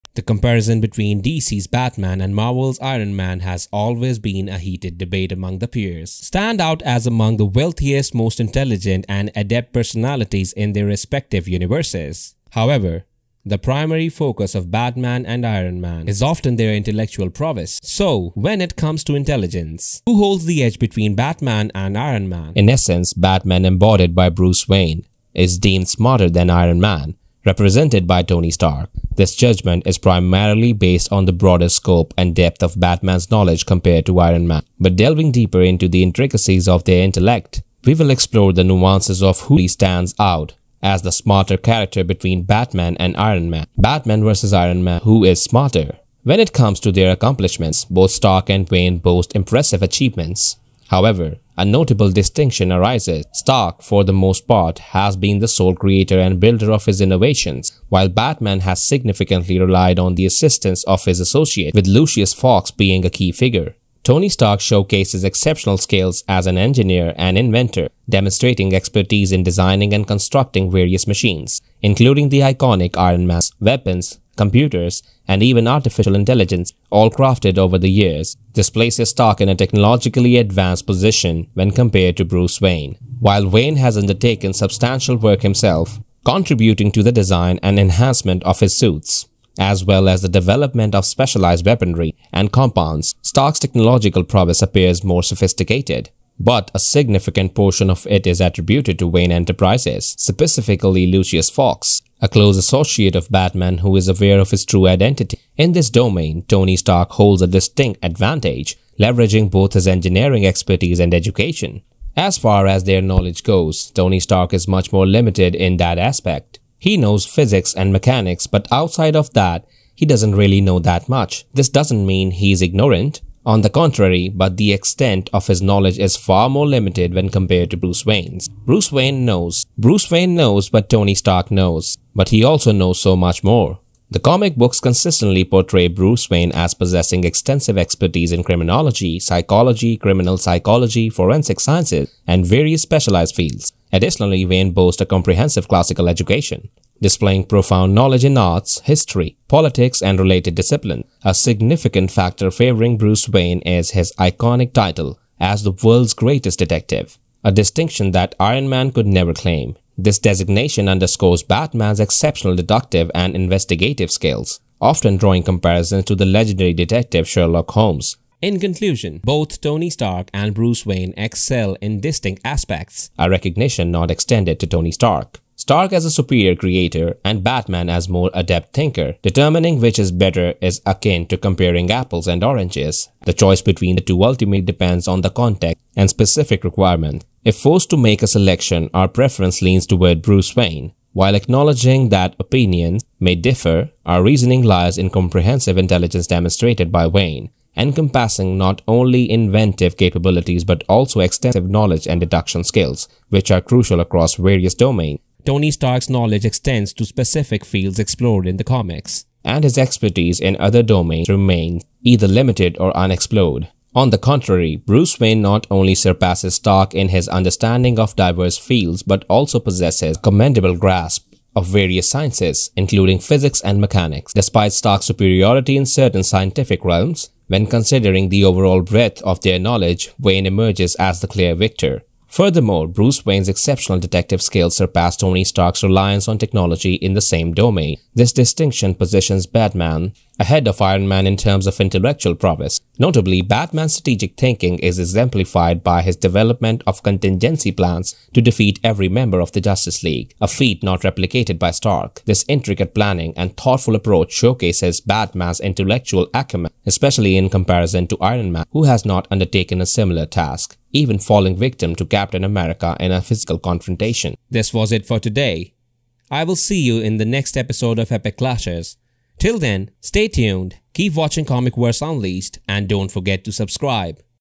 Batman VS Ironman | Who is smarter?
Description: Delve into the intellectual prowess of Batman and Iron Man, two iconic superheroes known for their exceptional intelligence. Explore the realms of detective skills, engineering genius, and strategic thinking as we analyze who holds the title of the smarter superhero. Join us in this cerebral showdown between the Dark Knight and the Armored Avenger.